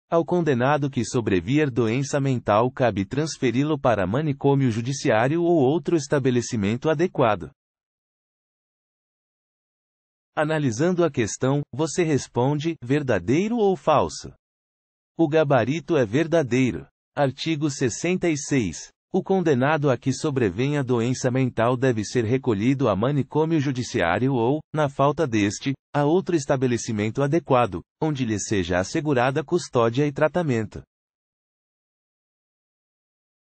QScon Direito: [Art. 66, CPM] MPM 1999 - Promotor de Justiça Militar
Art. 66, CPM | MPM 1999 | Promotor de Justiça Militar

Ao condenado que sobrevier doença mental cabe transferi-lo para manicômio. judiciário ou outro estabelecimento adequado.

Gabarito: Verdadeiro